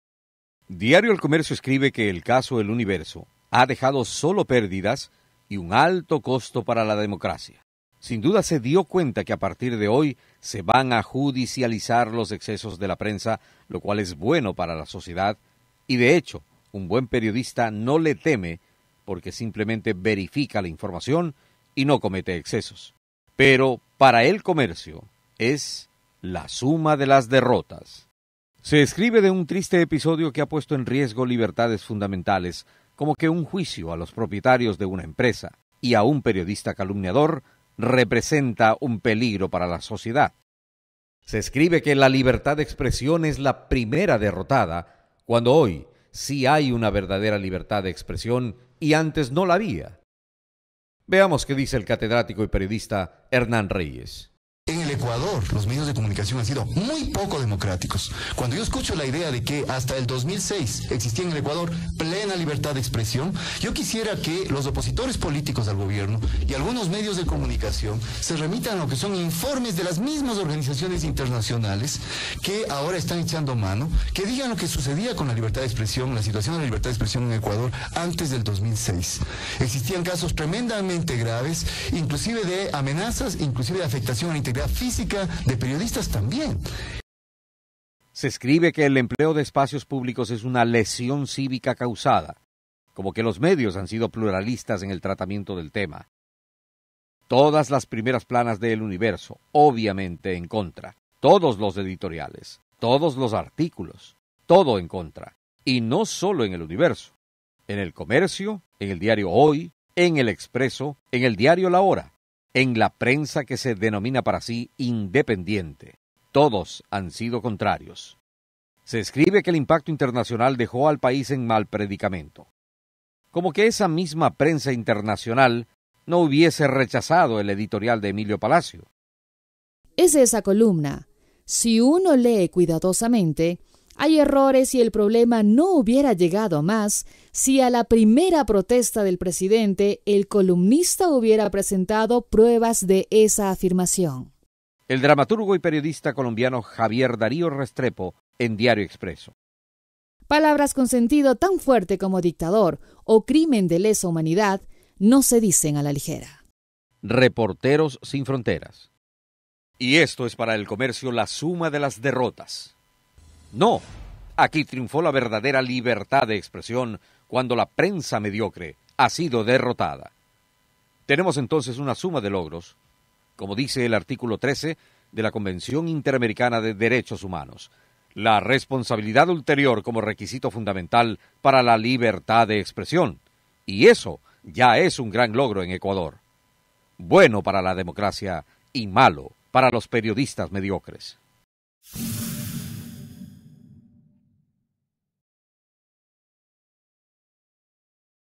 Supuestos medios independientes se alinean contra Gobierno
El Comercio, en el editorial "La Suma de las Derrotas", defiende los artículos diario El Universo por acusaciones que hace en contra del Presidente de la República, Rafael Correa, sin verificar ni contrastar datos.